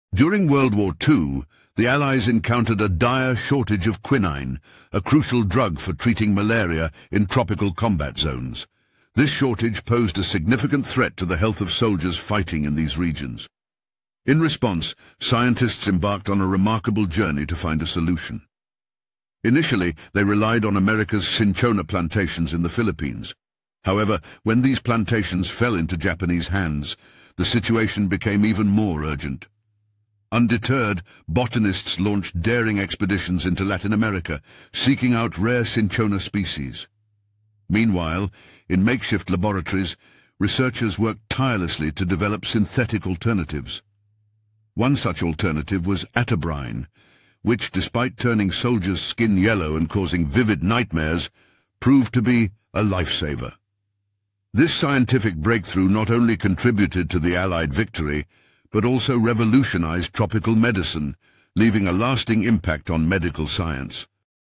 How Malaria Nearly Defeated the Allies in World War 2
🌿 When malaria threatened Allied troops in WW2, scientists waged their own battle against disease! This video explores how researchers raced to find quinine alternatives after losing Philippine cinchona plantations, leading to breakthrough medications like Atabrine. Despite turning soldiers yellow and causing nightmares, these treatments saved countless lives across Pacific and African theaters. Discover the untold scientific mission that helped secure victory!